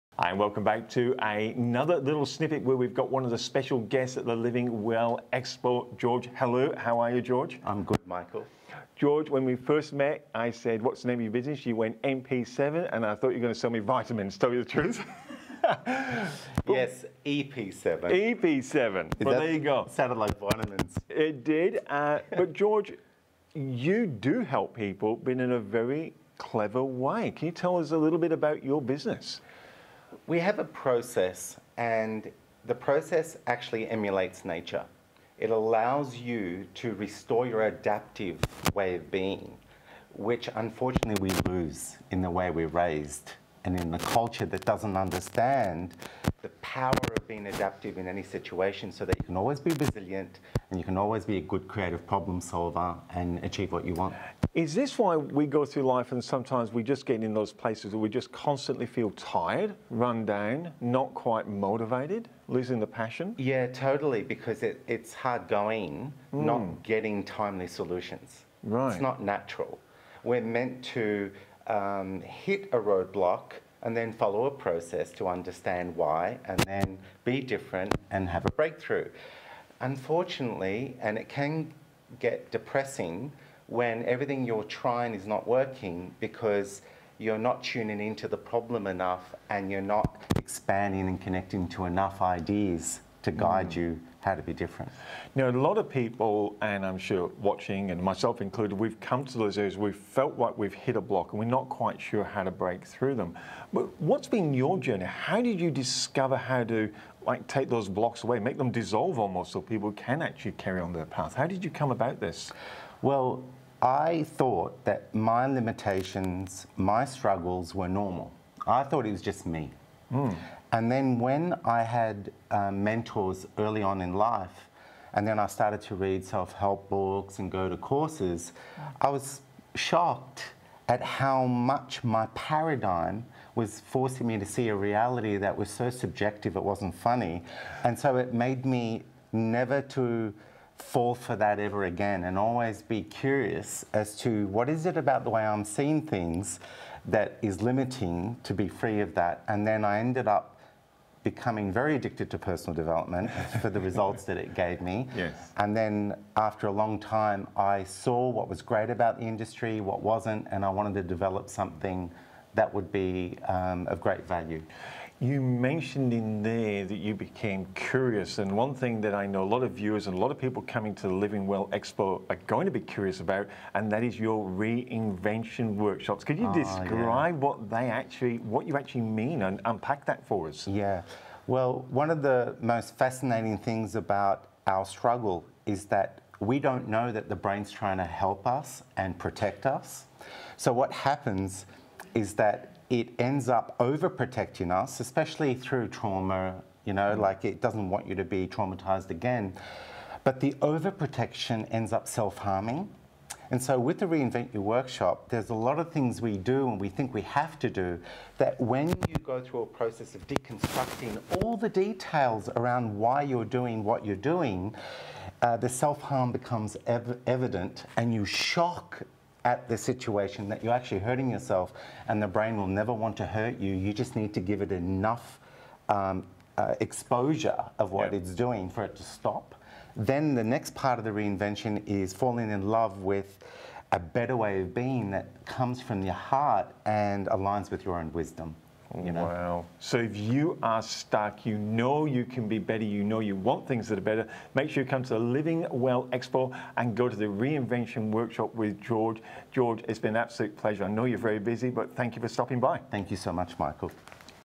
Restore your adaptive way of being.
George Helou is a best selling author, life coach and the founder of EP7 – Empowered for Purpose in 7 Steps. He is a husband, father and his hobbies include writing and music composition. His latest book is “Cinderella’s Secrets” – The Untold Story of Ella, launched at the Perth Town Hall by the Rt Hon Lord Mayor of Perth, Lisa-M Scaffidi. George’s upcoming book is “Empowered for Purpose in 7 steps”.

Appearing on the main stage and talks theatre at Living Well in WA - Solutions for Life - May 18 & 19.